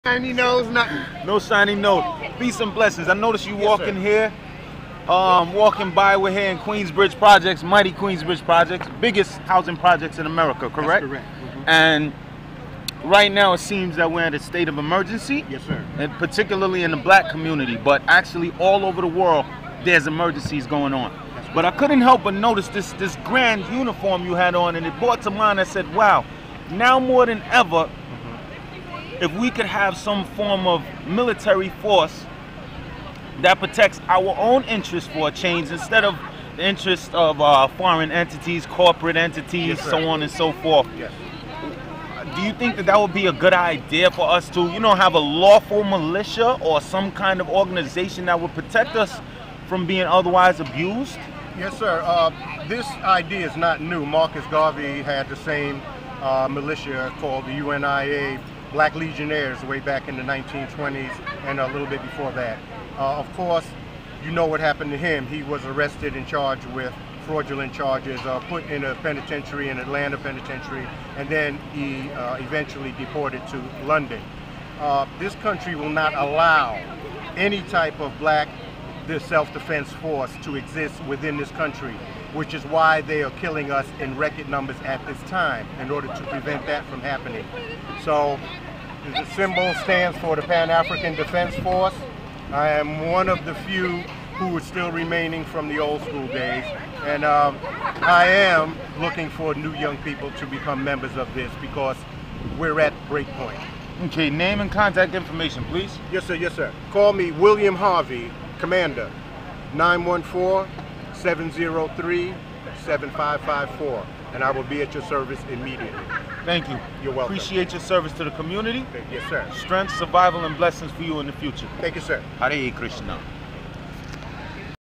PAN AFRICAN DEFENSE FORCE
A brief interview with a person who had on an interesting uniform. His essence is quite appropriate in light of recent and chronic race problems in America. Presenting, Commander William Harvey.